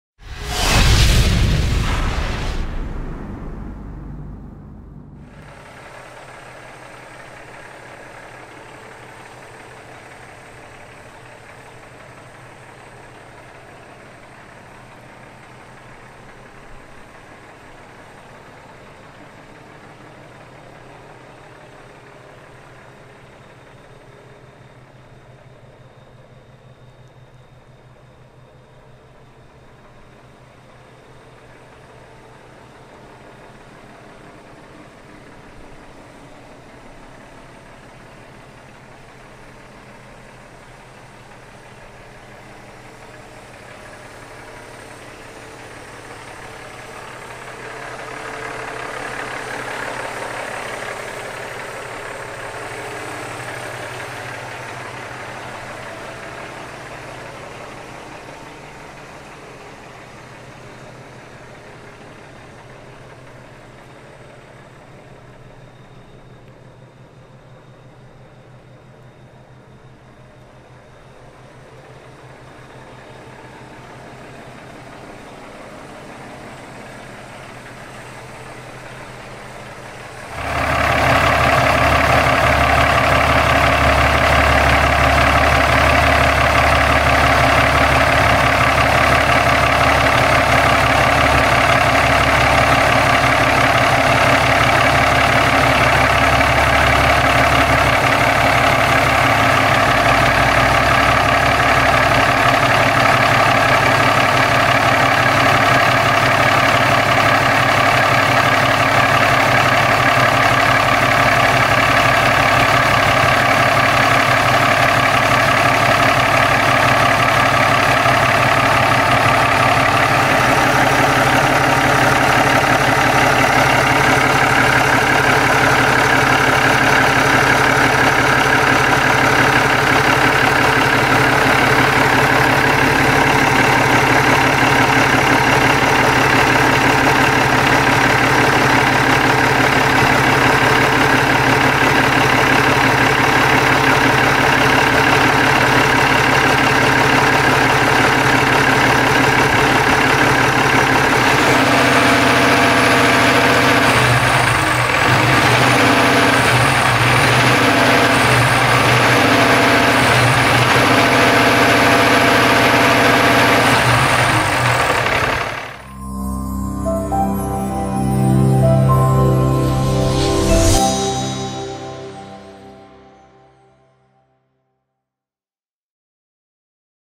2007 PETERBILT 387 SEMI-TRUCK FOR SALE / CAMION À VENDRE
2007 PETERBILT 387 WITH A CUMMINS ISX 15L DIESEL ENGINE , 400 HP , 13 SPEED EATON-FULLER TRANSMISSION , 12.000 LBS FRONT AXLE , DANA SPICER 40.000 LBS FULL LOCK , RATIO 3:29 , 275/80R22.5 TIRES AT THE FRONT , 11R22.5 AT THE BACK , ALL ALUMINIUM WHEELS , A THERMO-KING TRIPAC , A 2550 WATTS COBRA POWER INVERTER AND A FRIDGE